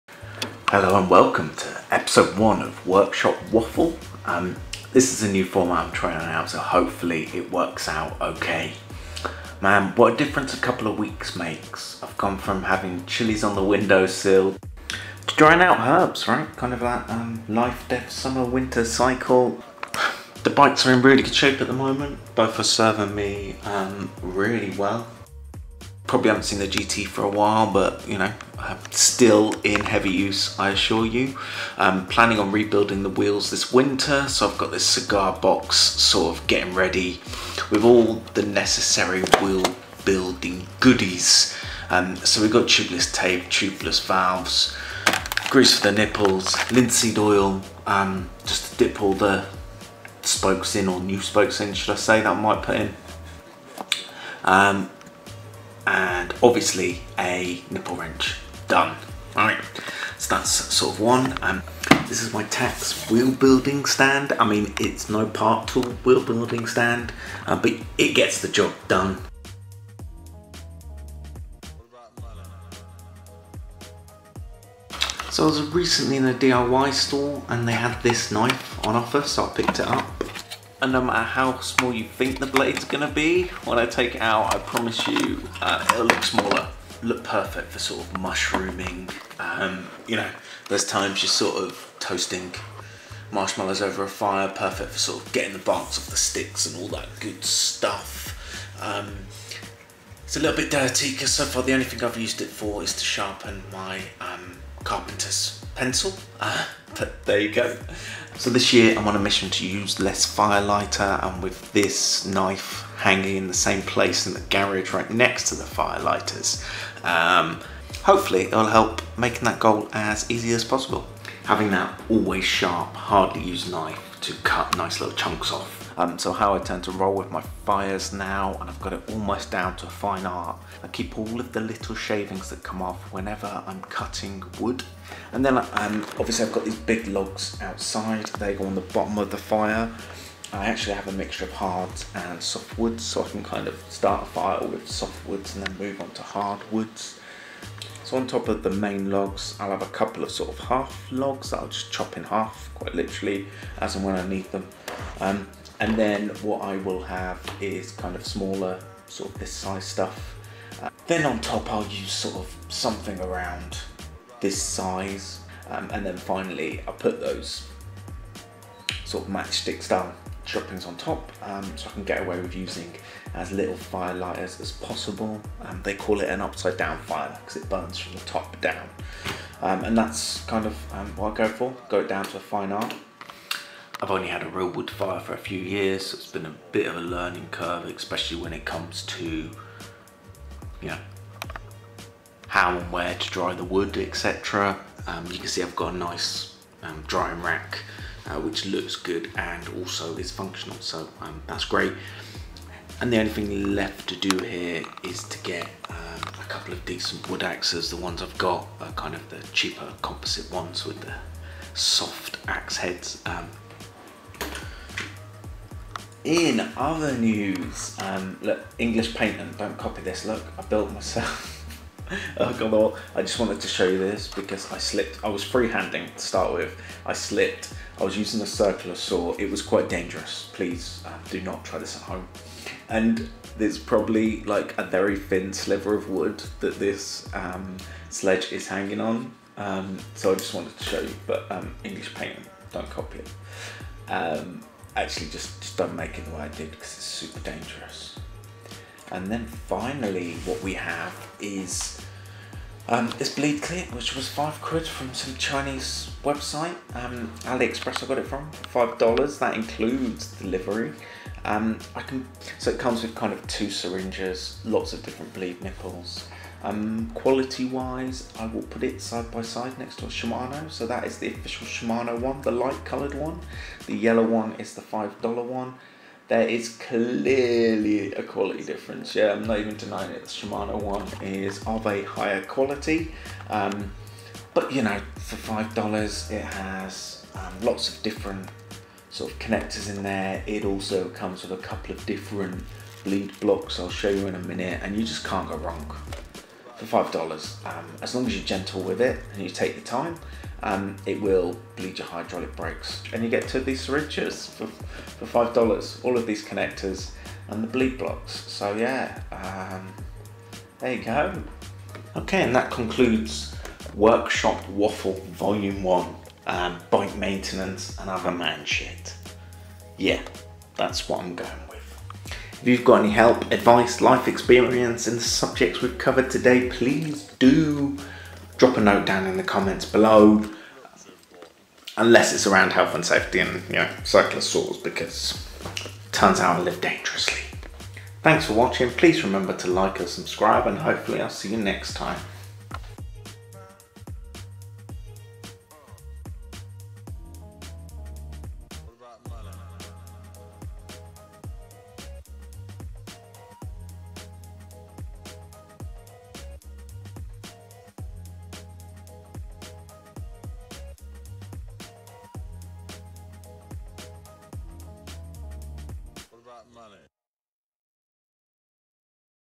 Workshop Waffle Vol 1 - Bleed kits, firewood and a custom sledge hook
In todays workshop waffle bike stuff, firewood, custom hooks, new knife and more!